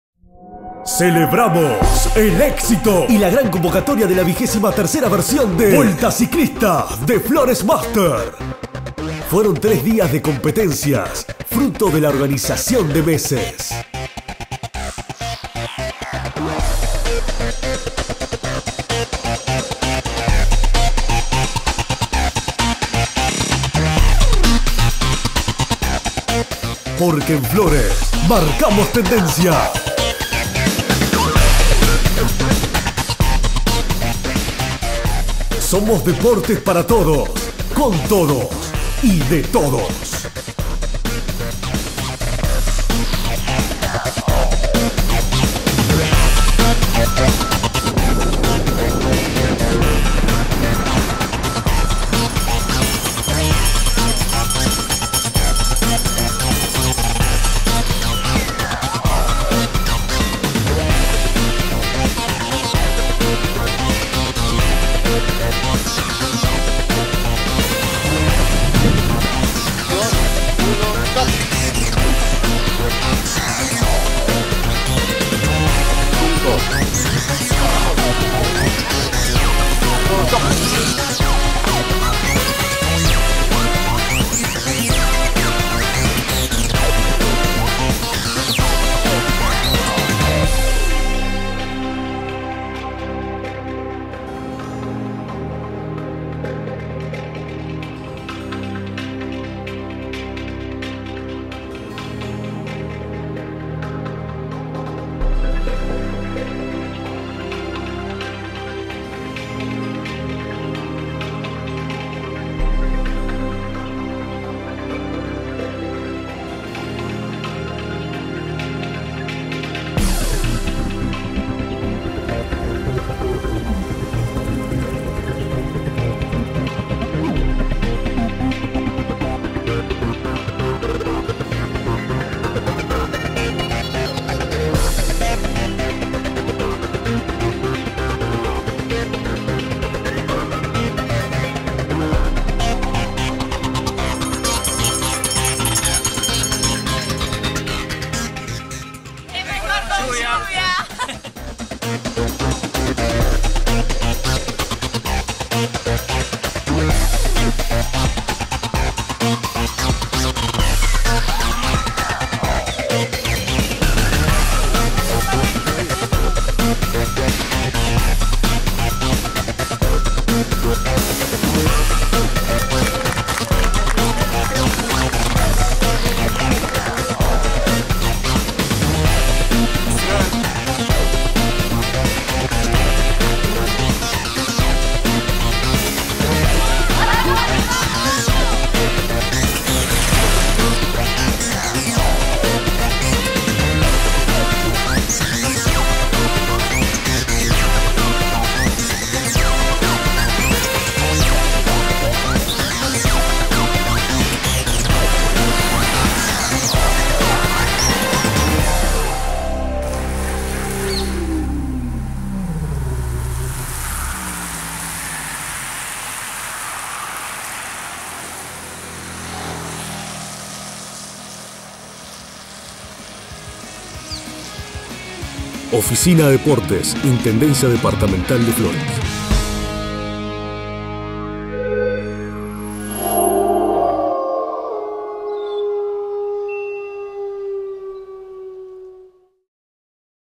23ª Vuelta Ciclista Flores Master 2017
Disfruta con nosotros la 23ª Vuelta Ciclista Flores Master 2017. En 4 minutos sabrás como se vivió el deporte en Flores. Deporte para todos, de todos y con todos.